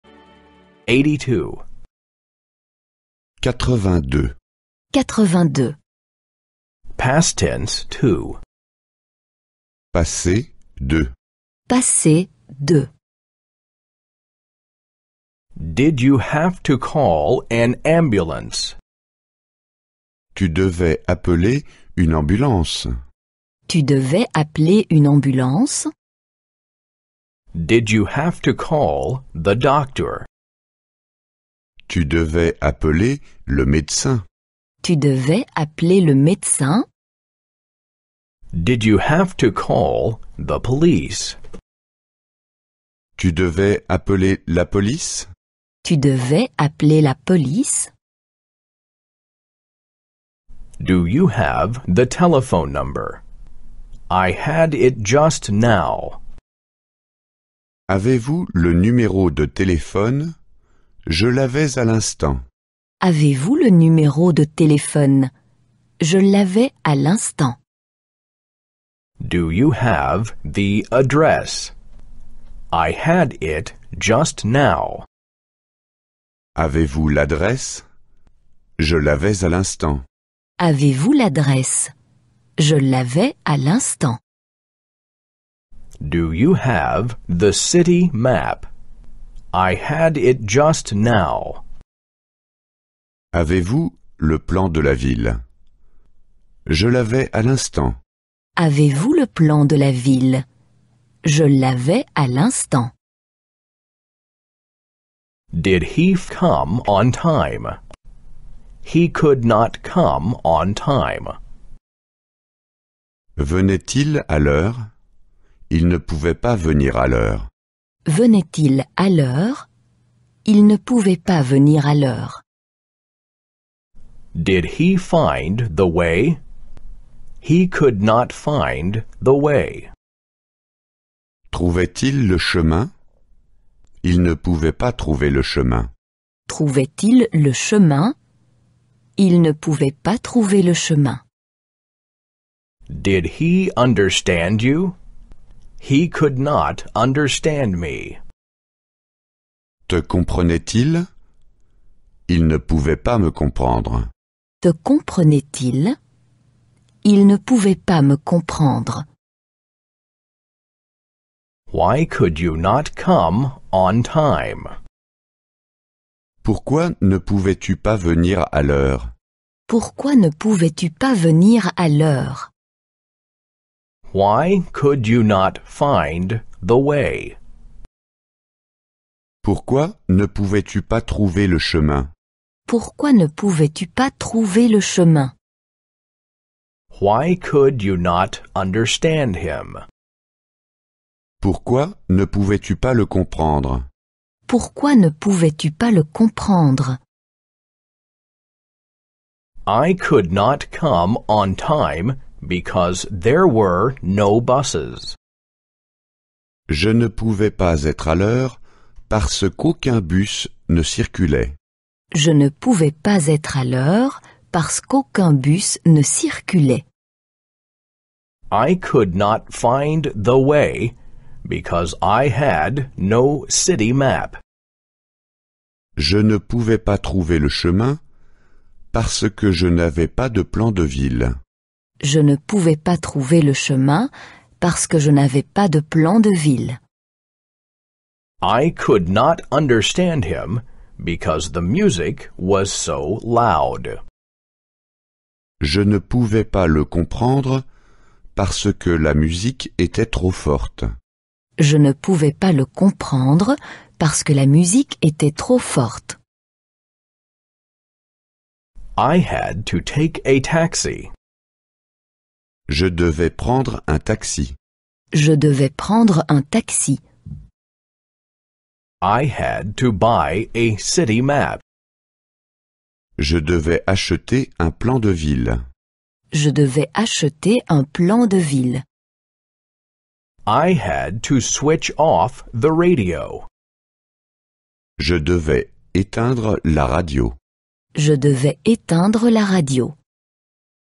All French in 100 Lessons. Learn French. Most important French phrases and words. Lesson 82
If you want to start learning French, this video is for you.

In this video, we are going to look at one of the lessons in our series "All French in 100 Lessons". Learn the most important French phrases and words to improve your French skills quickly and easily!

From basic greetings to everyday expressions, these lessons will help you expand your vocabulary and improve your overall understanding of the French language. Whether you're a beginner or just want to brush up on your French, this series is a great resource for language acquisition. Merci!